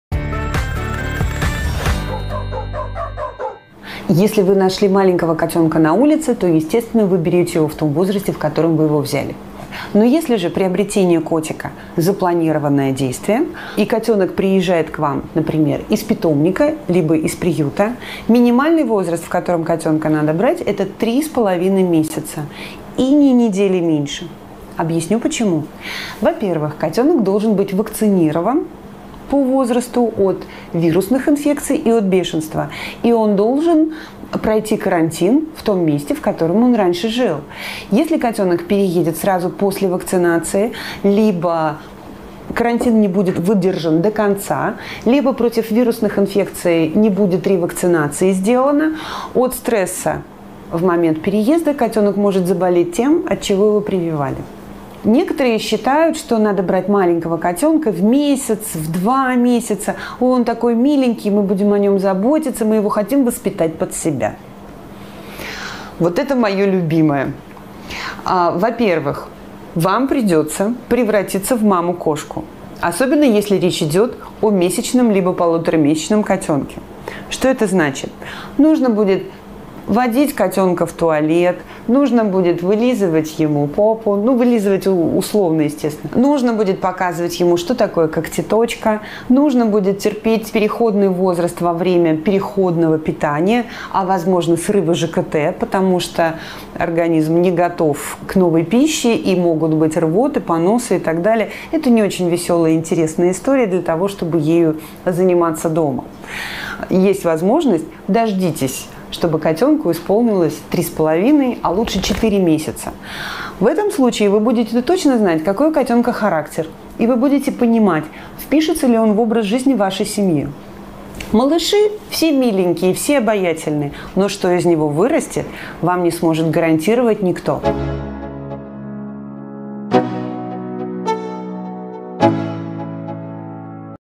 В каком возрасте брать котенка с приюта или питомника? Советы фелинолога
Многих интересует вопрос: в каком возрасте рекомендуется забирать котенка с приюта или с питомника? На этот вопрос отвечает фелинолог.